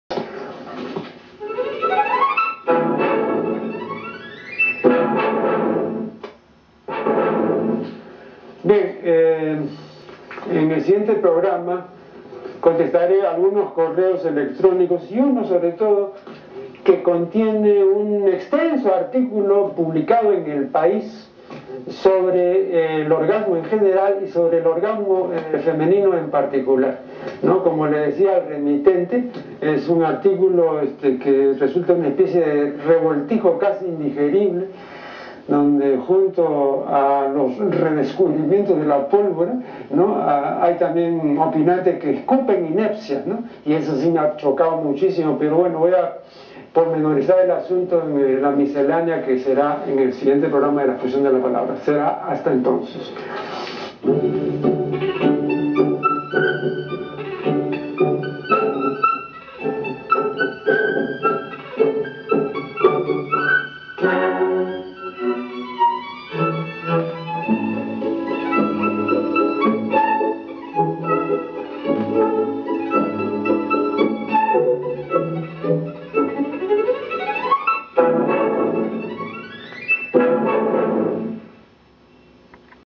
LA FUNCION DE LA PALABRA(Georgette Vallejo al fin de la batalla)6ta parte
PRESENTACION DE LA OBRA GEORGETTE VALLEJO, AL FIN DE LA BATALLA POR EL PROFESOR MIGUEL PACHAS ALMEYDA . LIMA - 2008